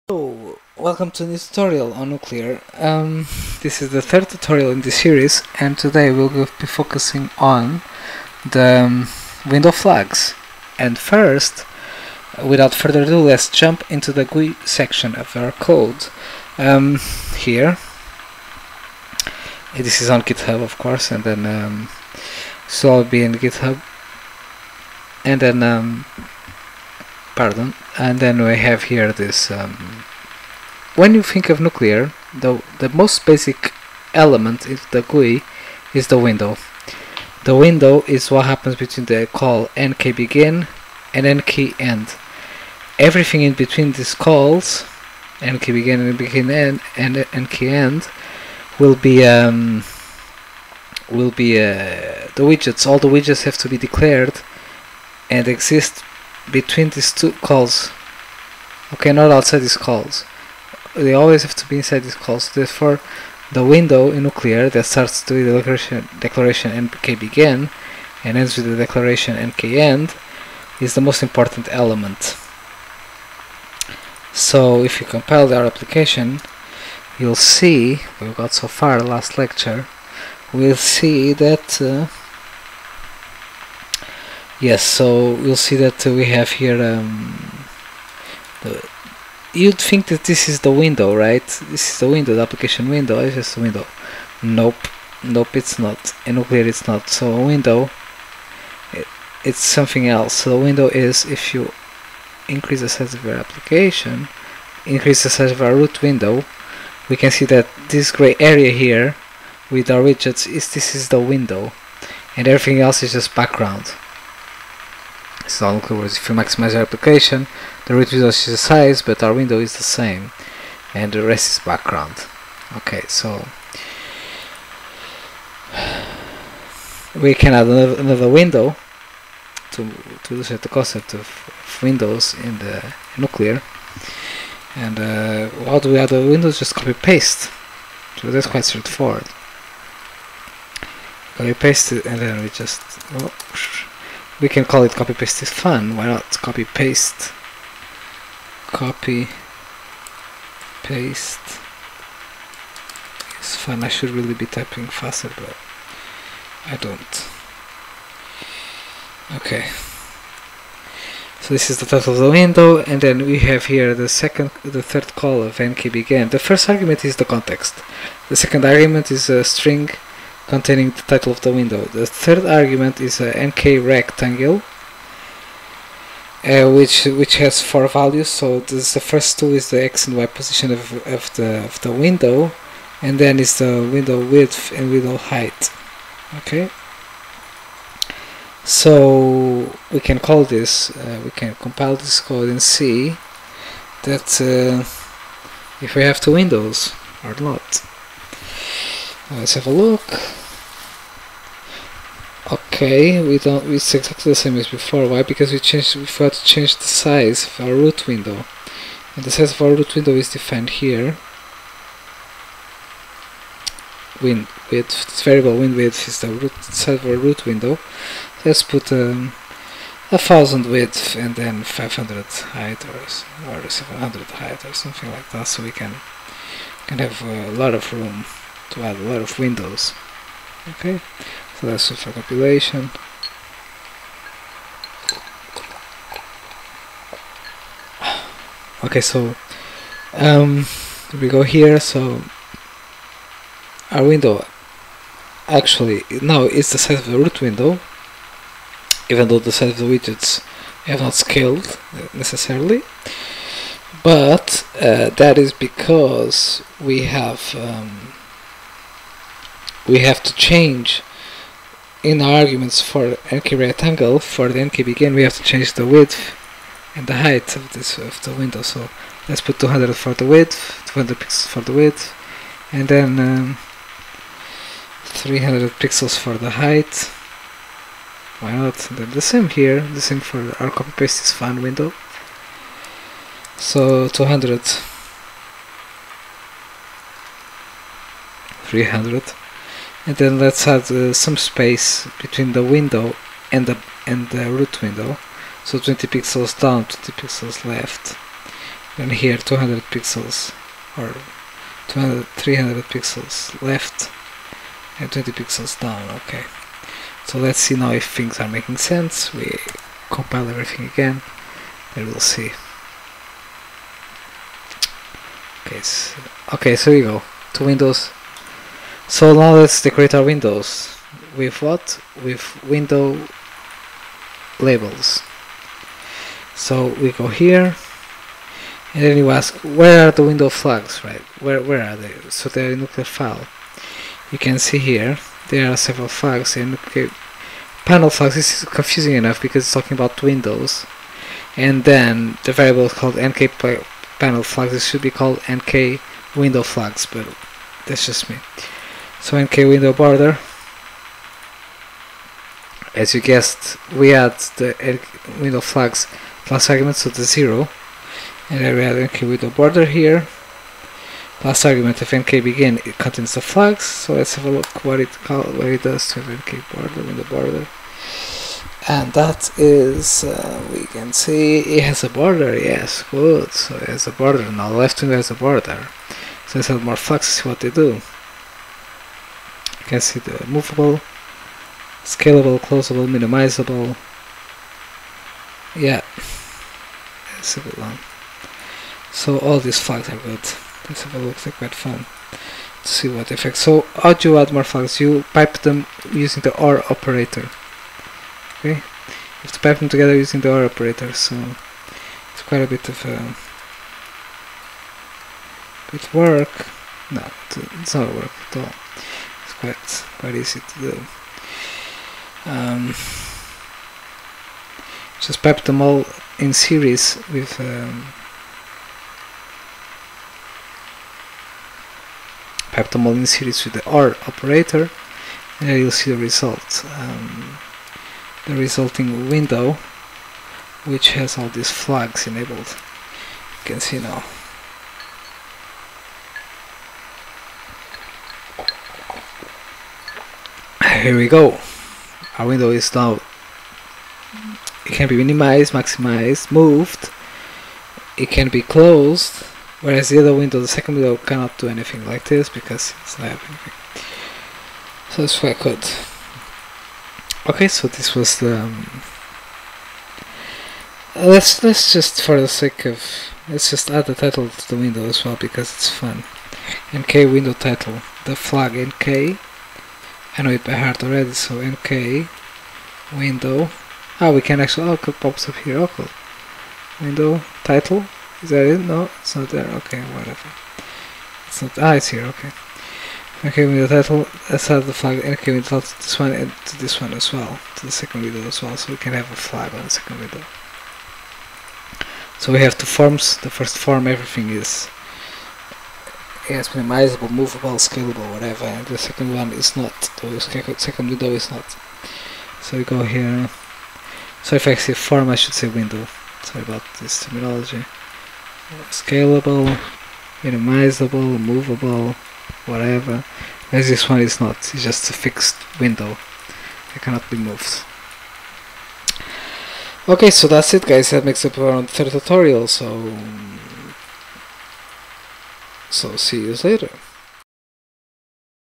Nuklear Tutorial 2 - Nuklear Windows
Hi, we go over the concept of a Nuklear window and respective window flags.  Hope this is helpful. Happy hacking!

SuperPepperMint are now offering online workshops on Nuklear.